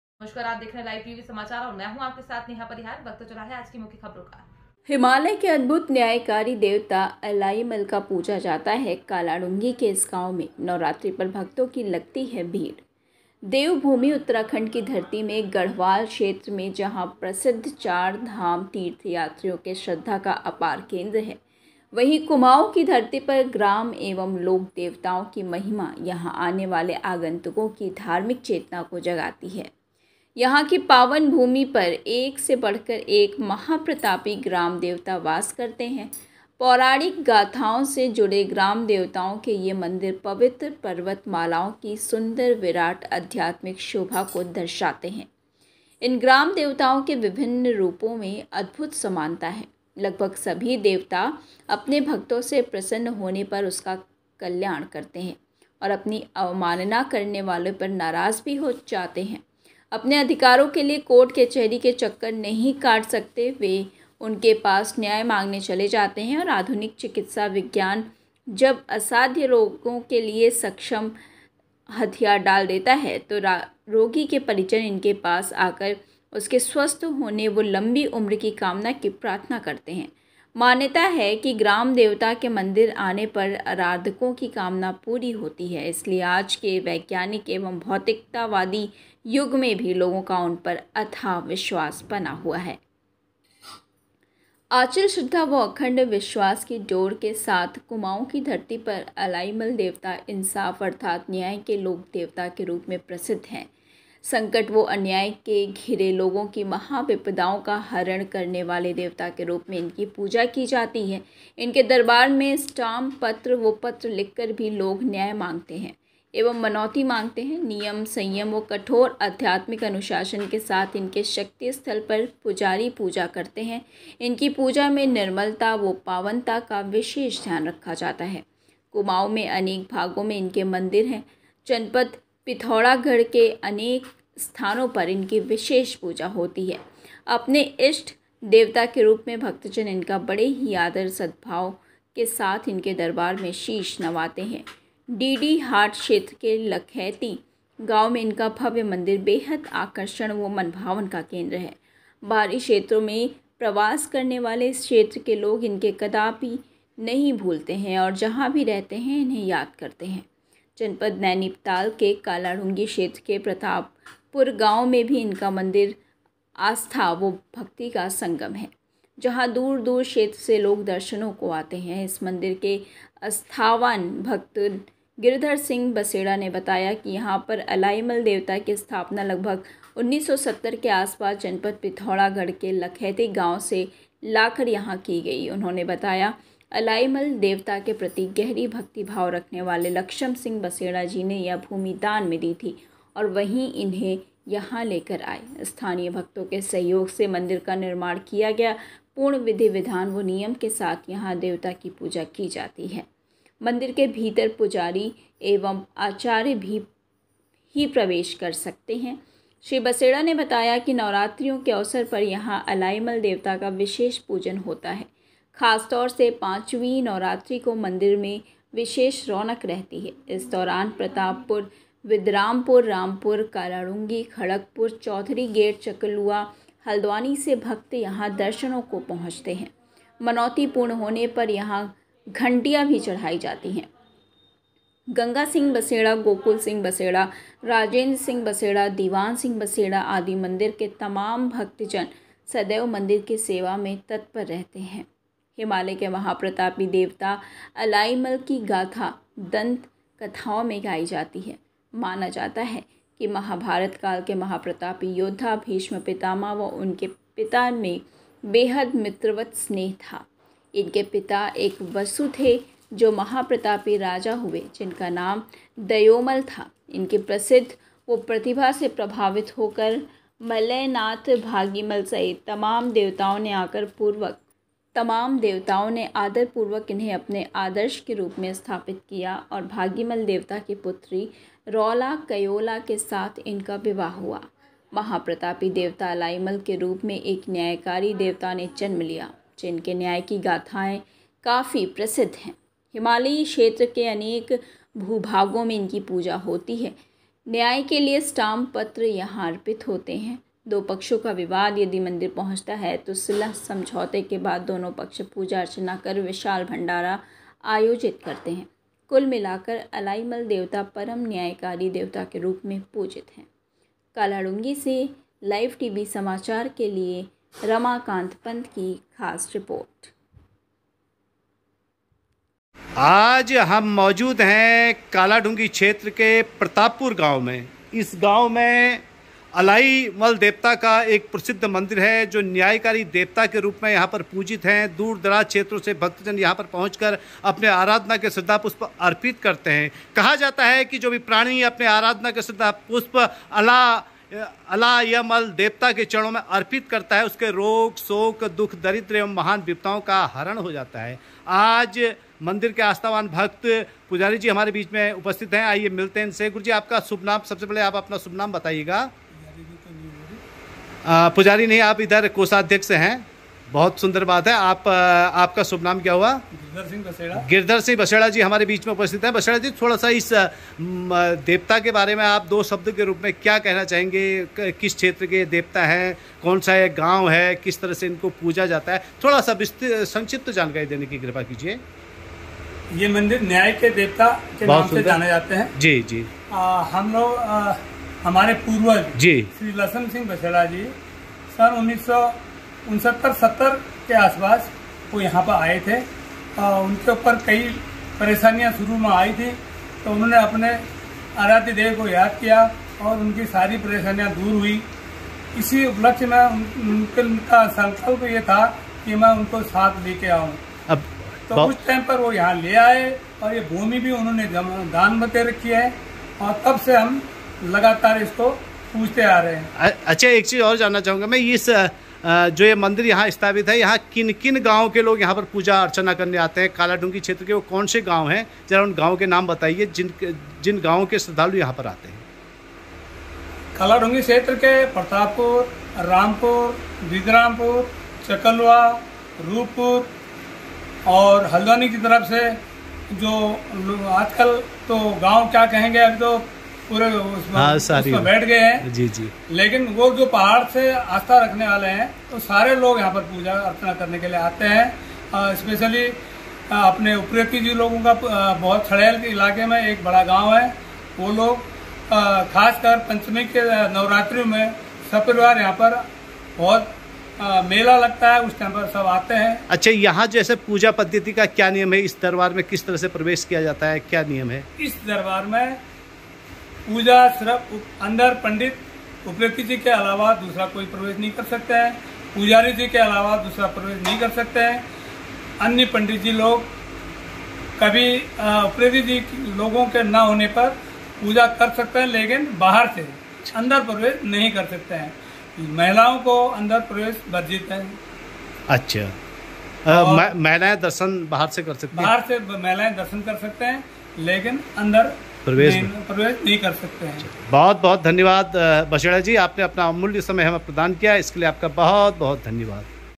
कालाढूंगी : हिमालयके अद्भूत न्यायकारीदेवता अलाईमल को पूजा जाता है नवरात्री पर भक्तों की लगती है भीड़
कालाढूंगी : हिमालय के अद्भूत न्यायकारी देवता अलाईमल को पूजा जाता है नवरात्री पर भक्तों की लगती है भीड़